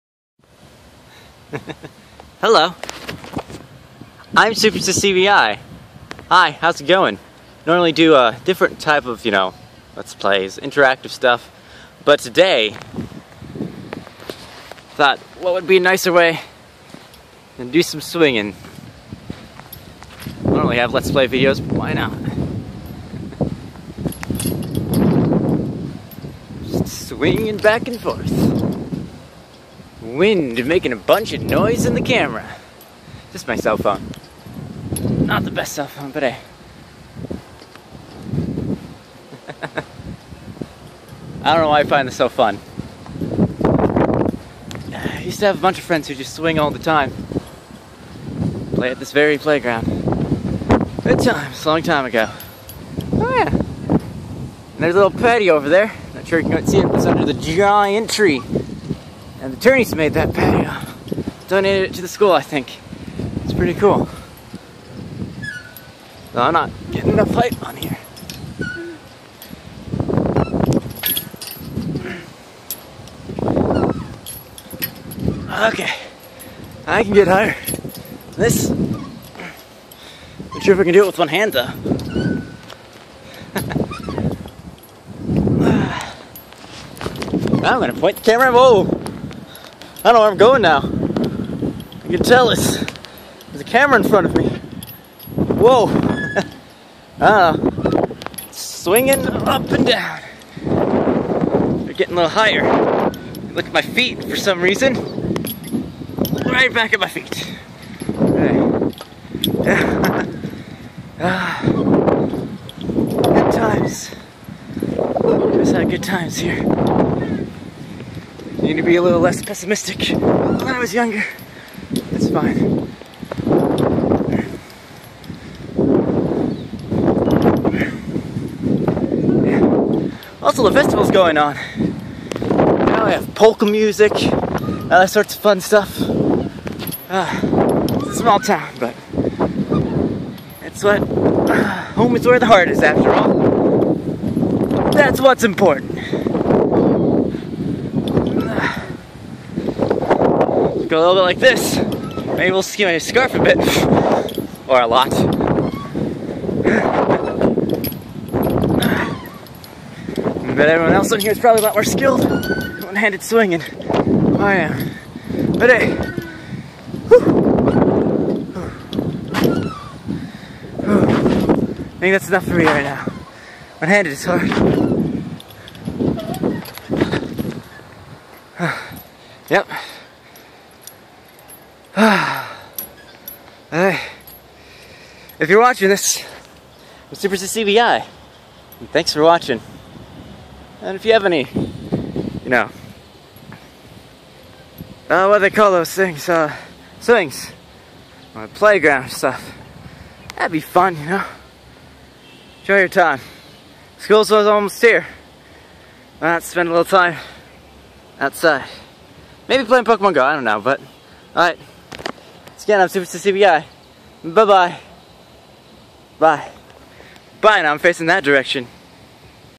3 Minutes of Swinging @ All Saints Festival
Yep! I just spend 3 minutes recording myself swinging. Kinda difficult to do it alone, but it was fun! :D I also can't remember all the stuff I said, but it's probably okay and family friendly, right?

Hope so! ^^ The festival is always a nice time to visit and remember all the old shenanigans you'd get into with your classmates. Ah. Good times.

(Also, people have been saying I should do more vlogs. ^^; My life isn't that interesting, but I'll try!)